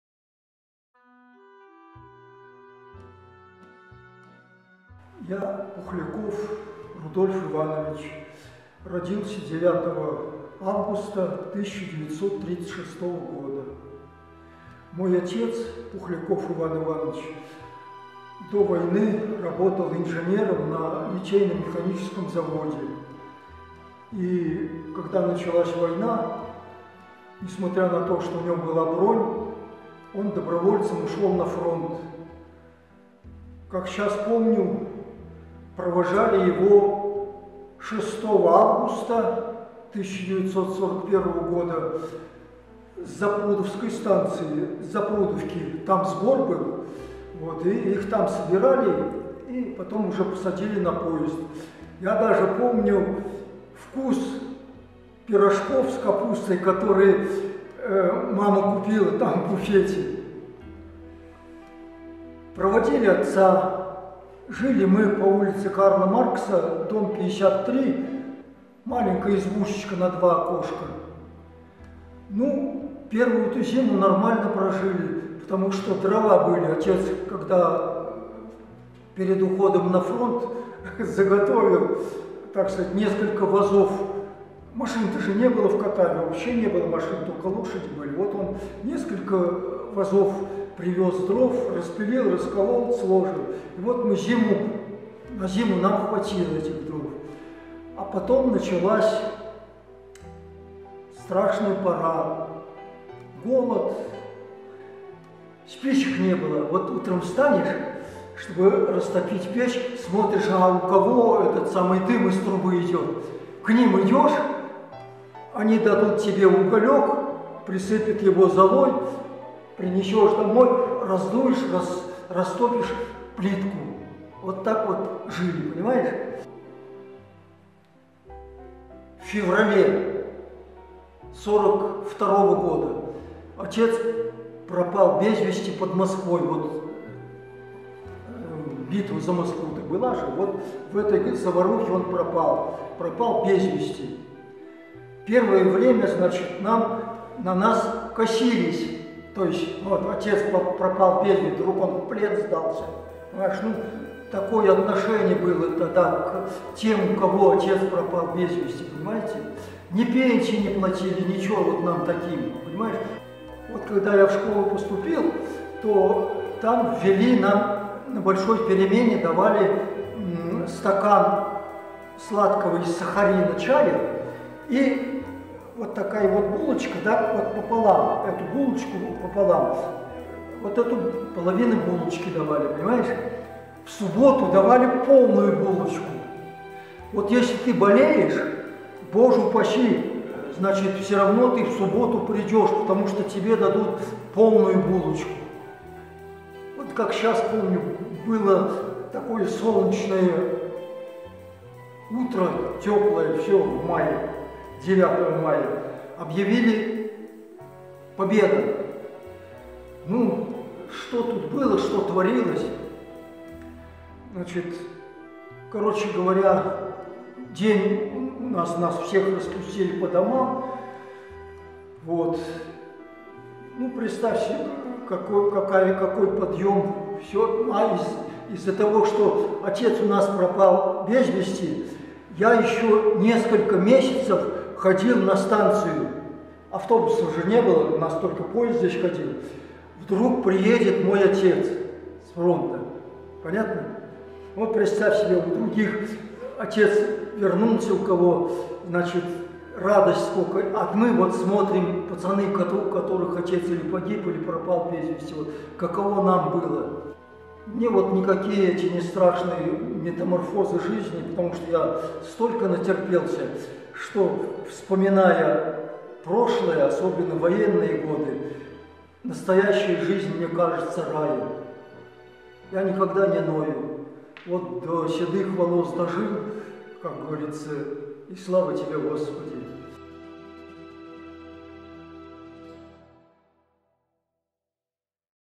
"Моё детство-война".Пухляков Рудольф Иванович Челябинская обл. Катав-Ивановский р-н г.Катав-Ивановск
"Моё детство - война" – социальный проект по сохранению памяти о Великой Отечественной войне. Конкурсную работу предоставили участники: МУК "Краеведческий музей Катав-Ивановского муниципального района", Любимова Елена Александровна, Гулин Александр Александрович из города Катав-Ивановск Челябинской области. Герой конкурсной работы - Пухляков Рудольф Иванович.